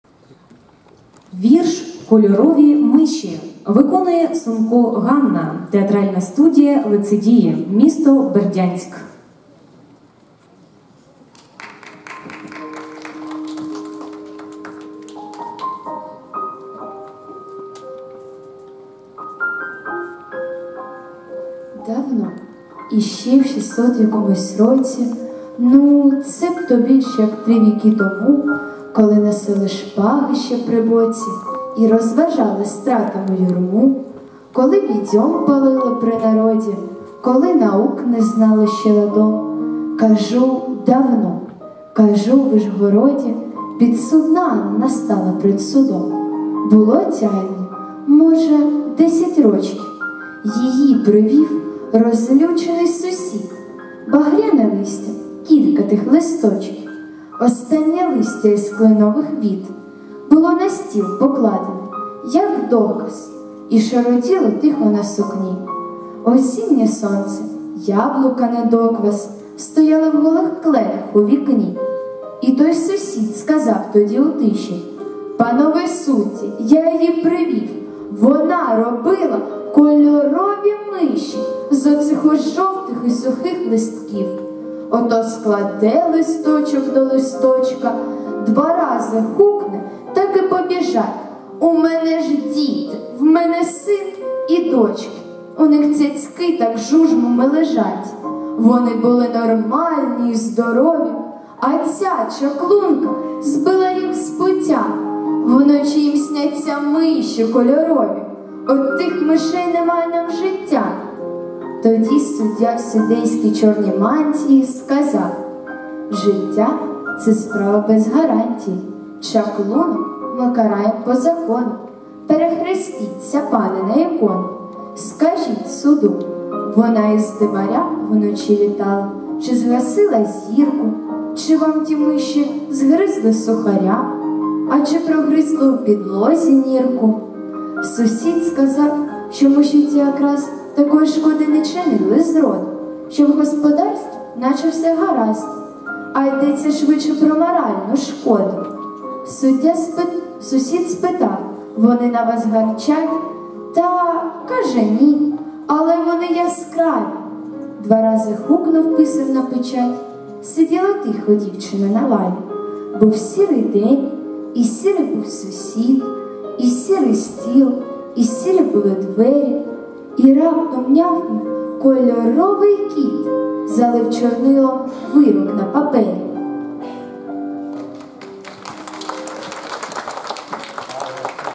Всеукраїнський фестиваль дитячої творчості «Топ-Топ»(День 2) (Частина 46)
Вірш "Кольорові миші"-Сунко Ганна